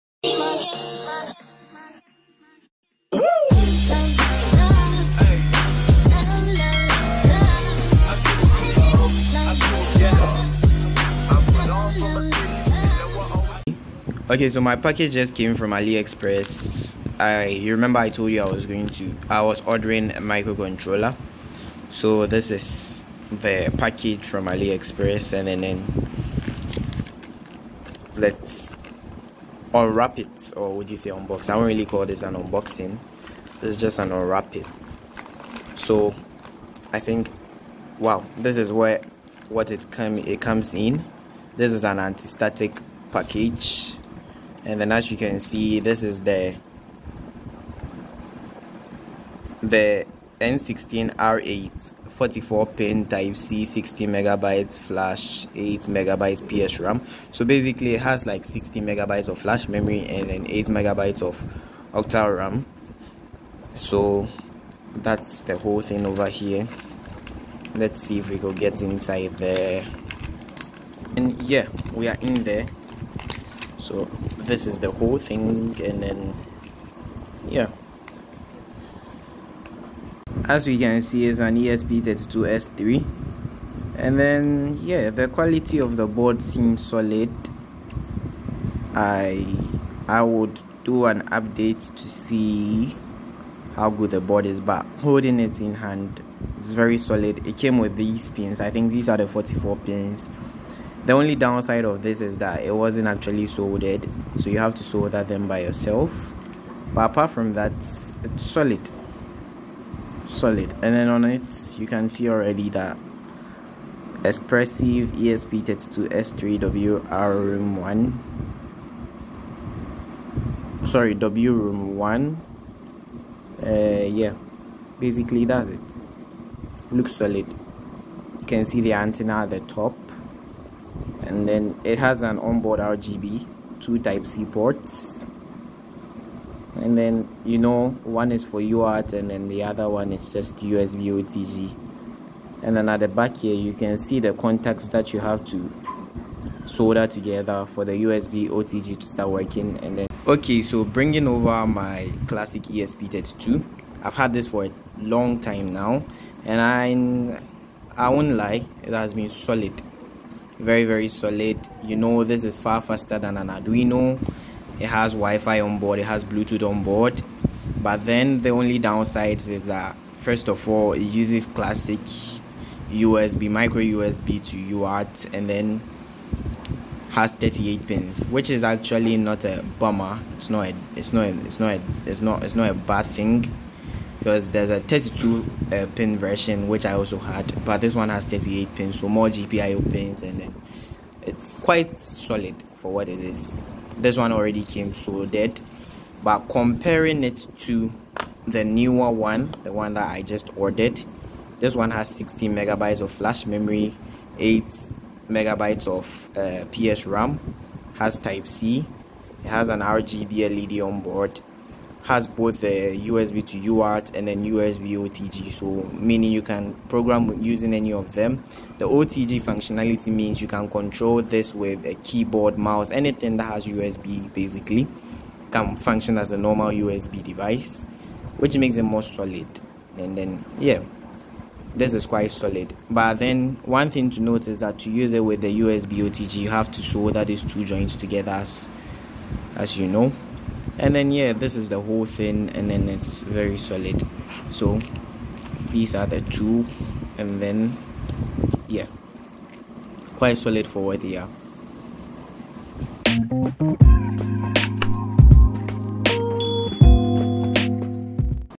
Unboxing and first Impression of esp32 s3 from Aliexpress#iot
A quick Unboxing and first impression of my esp32 s3 n16r8 from aliexpress.